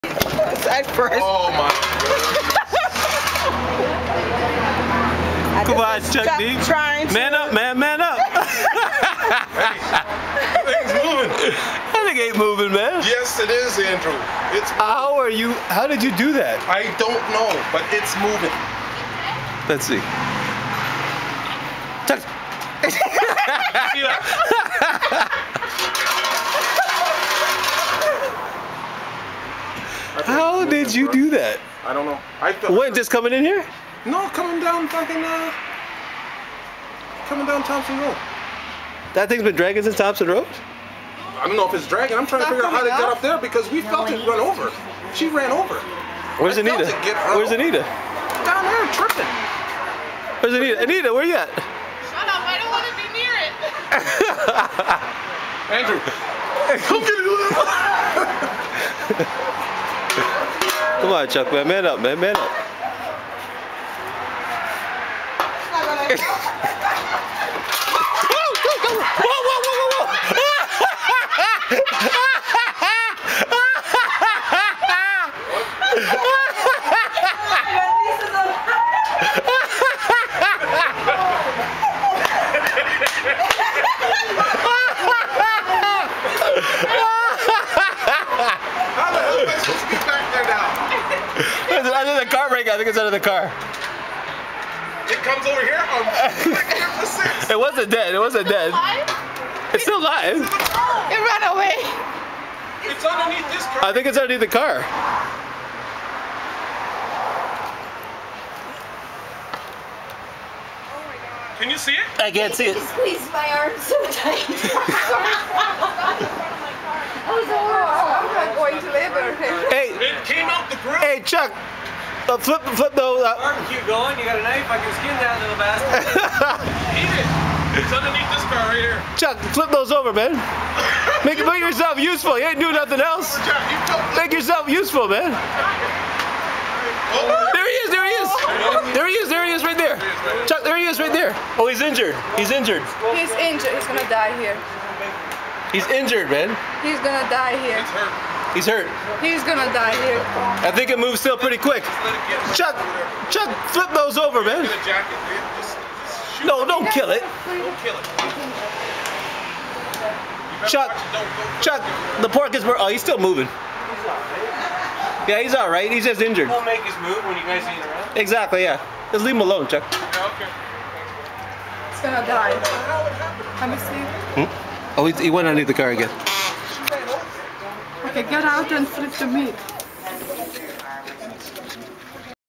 Chuck VS Squirrel
I was having a BBQ and told my friend Chuck to bring anyone he wanted, I guess the word got out to the local wildlife and the rest is history..My name is Andrew Harrington, and I approve this message.